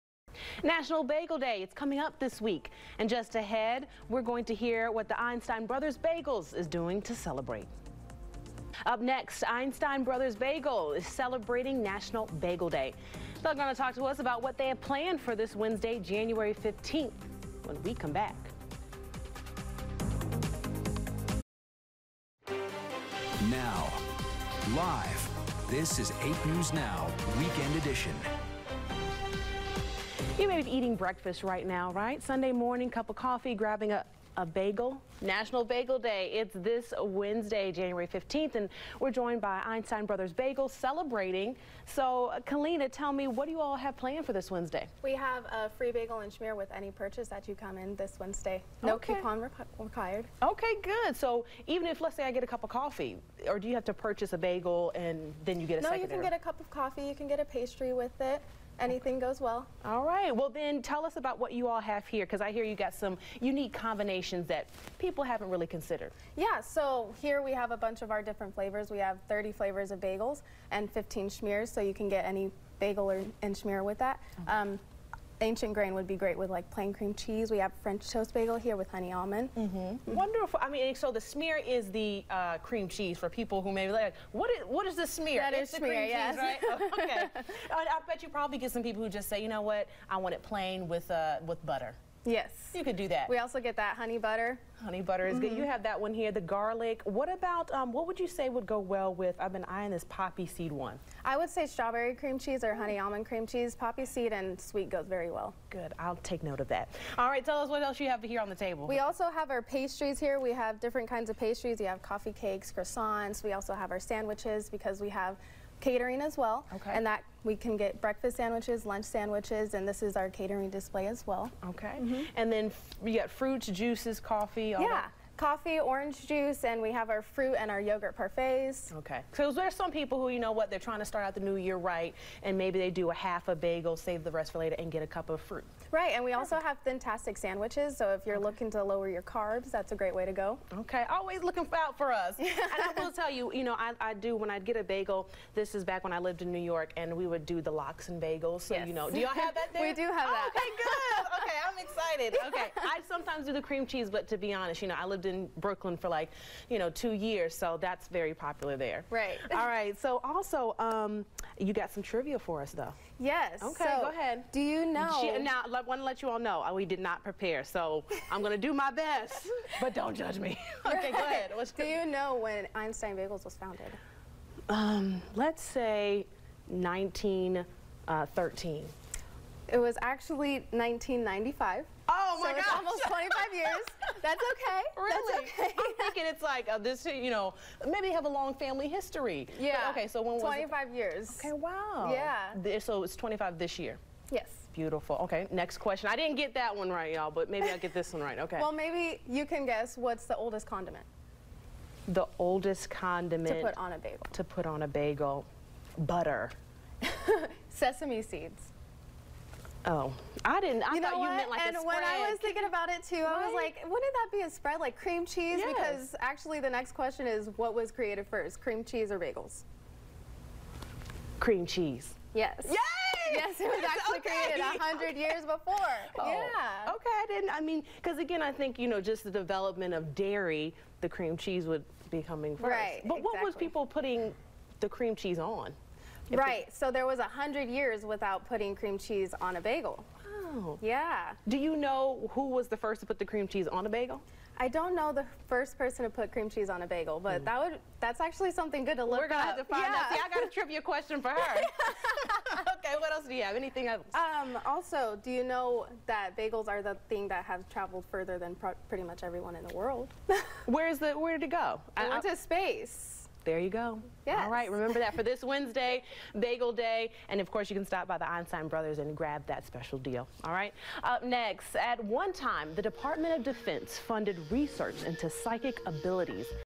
National Bagel Day 2020 Einstein Bros - Snackbox PR KLAS 8am
Customers across the U.S. can enjoy a FREE bagel with shmear on National Bagel Day, January 15th, 2020 at Einstein Bros. Bagels. Team Snackbox PR, based in Austin, TX, gains national coverage for Einstein Bros. Bagels for the 2020 holiday.